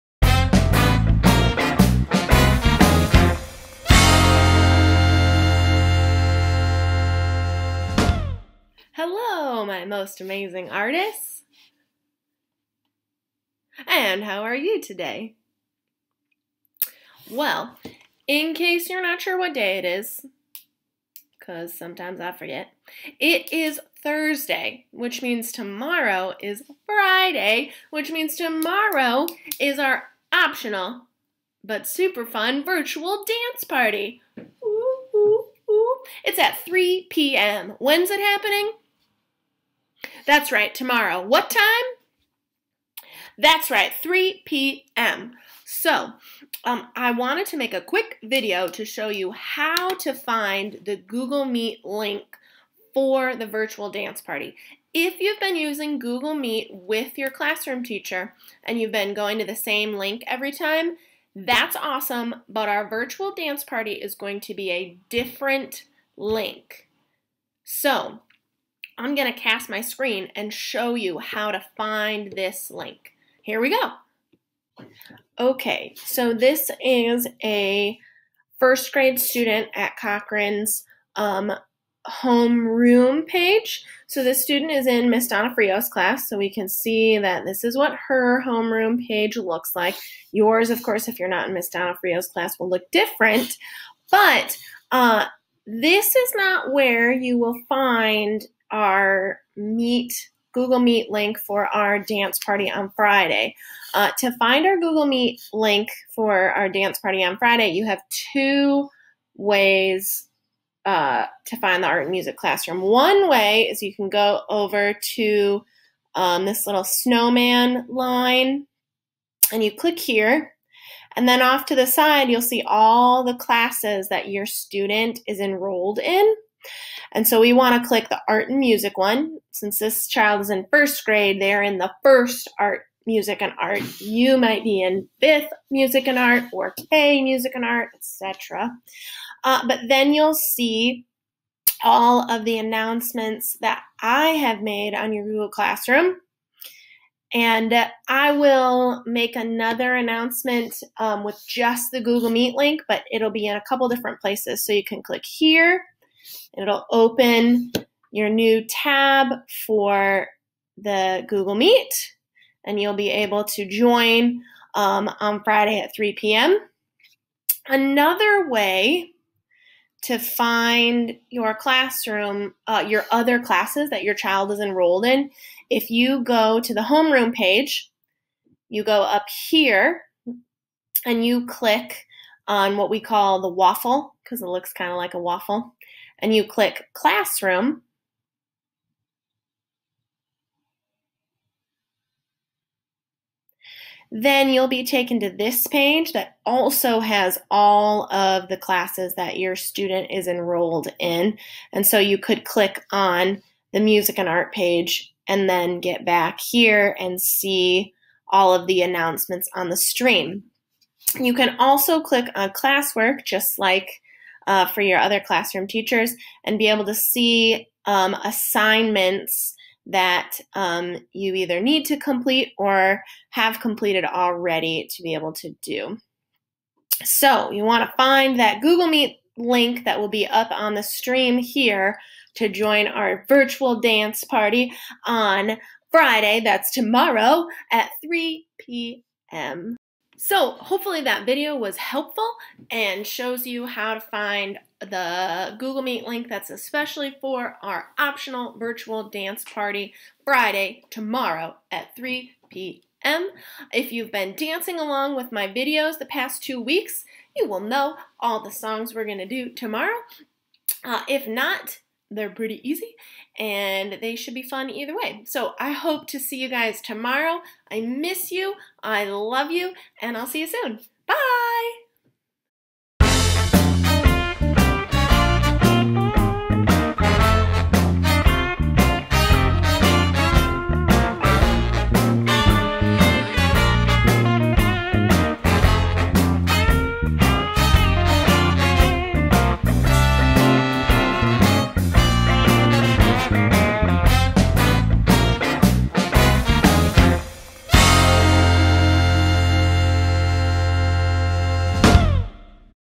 NTI Update Thursday April 23rd: Virtual Dance Party Friday @ 3PM!
Hello my most amazing artists!

In this video, Mrs. Collier will show you how to navigate between different Google Classroom classes and find the Google Meet link for our OPTIONAL live virtual dance party this Friday, April 24th at 3PM!

Let's create!

Be sure to check out Mrs. Collier's Instagram, @dontforgettocolliermama, and Twitter, @Mrs_K_Collier.